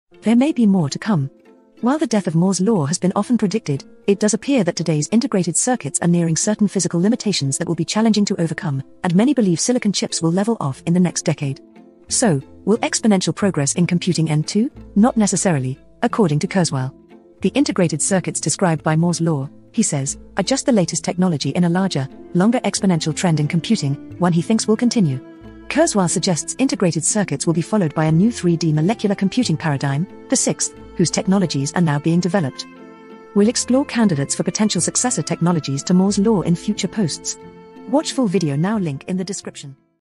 💻The Death of Moore's Law & the Future of Computing (Computer Science Debate)
Gordon Moore, one of the founders of Intel, made an observation back in 1965 that every two years the number of transistors that could be put on an integrated circuit would double. This observation came to be known as Moores Law and it has proven to be very reliable over the decades since its publication with transistors doubling roughly every two years . But if we look at our current computing power via Computer science and compare it to where we were just ten years ago we can see that this exponential growth will eventually come to an end, or so we believe. Will the end of Moores Law mean the end of exponential growth in computing?